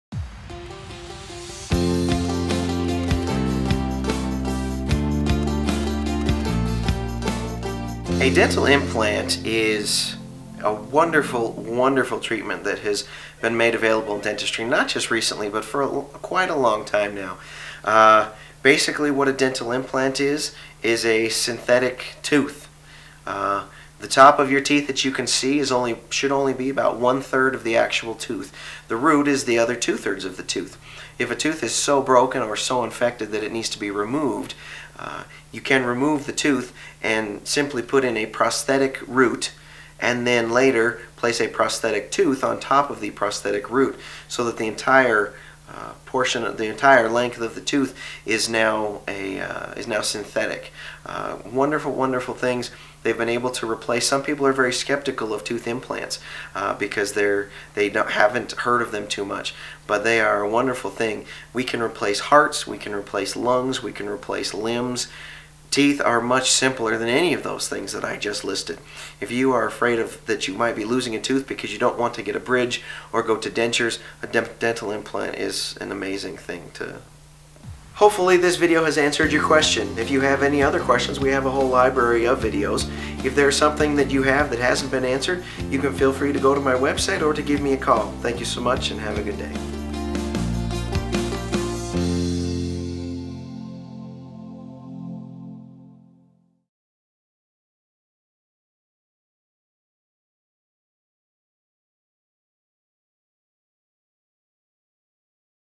Ogden Dentist - What is a dental implant?
Ogden dentist discusses dental implants.